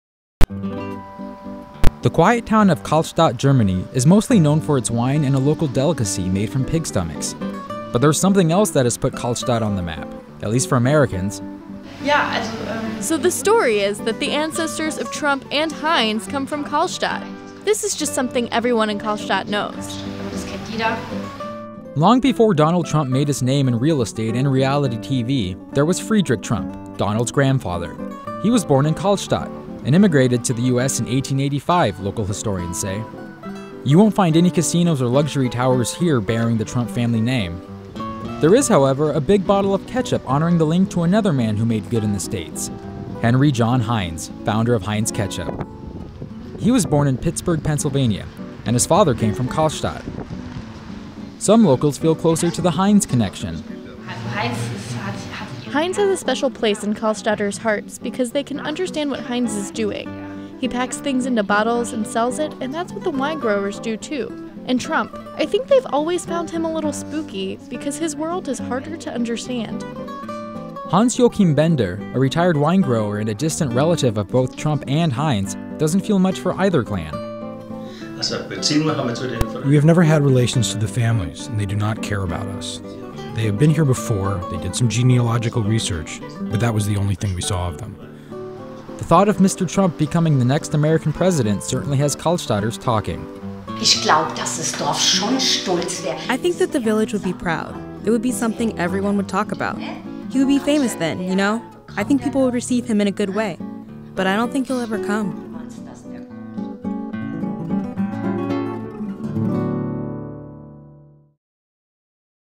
German Town Boasts Trump and Heinz Connections
Long before Donald Trump made his name in real estate, his grandfather immigrated to the U.S. from Kallstadt, Germany. The small town also has a connection to another famous American--Henry John Heinz, founder of Heinz Ketchup. Photo: Ellen Jervell/The Wall Street Journal